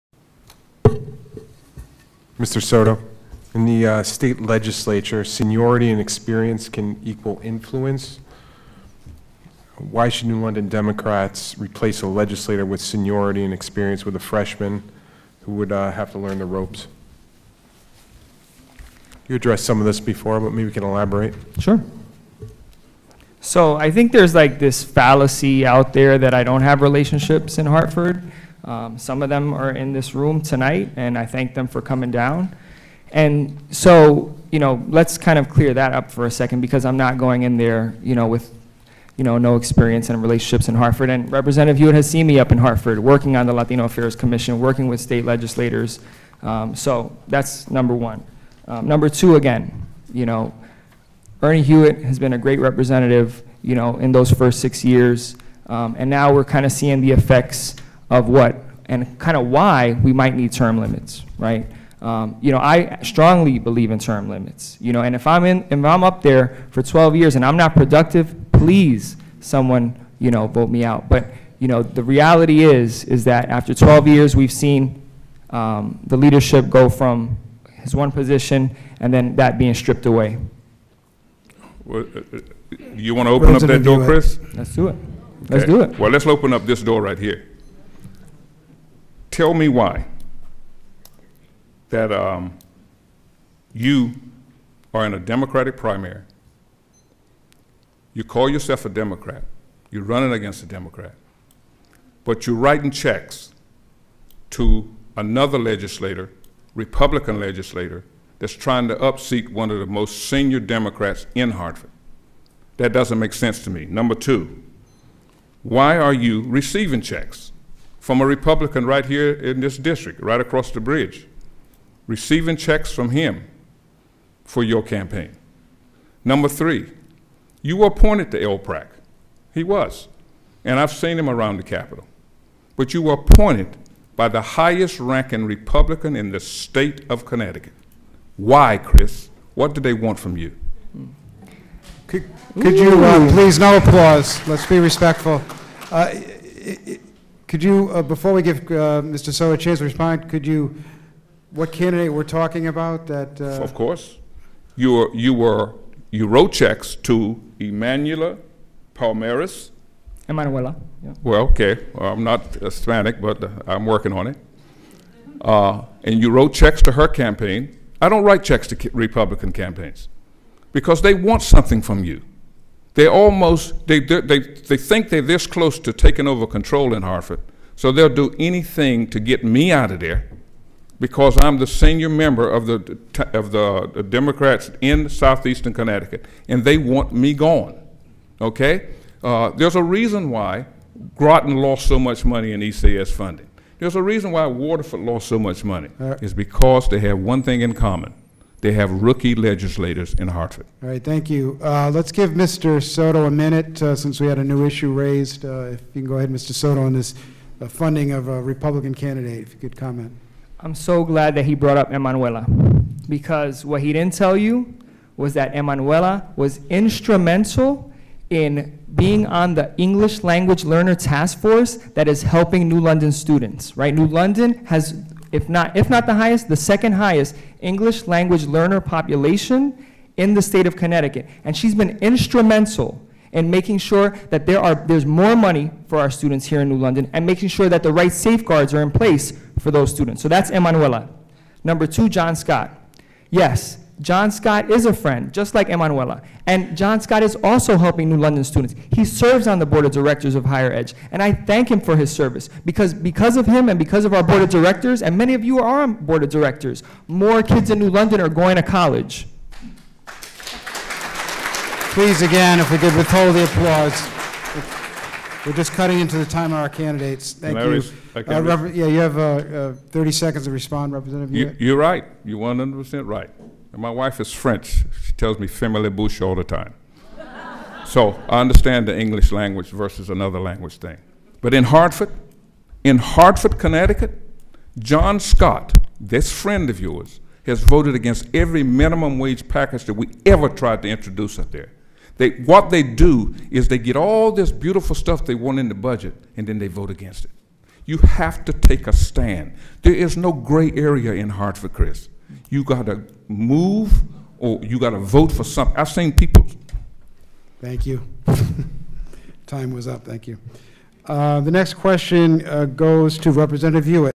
8. Hewett's seniority and Soto's relationships with Republicans
New London 39th House District Democratic Primary Debate: Rep. Ernest Hewett and Chris Soto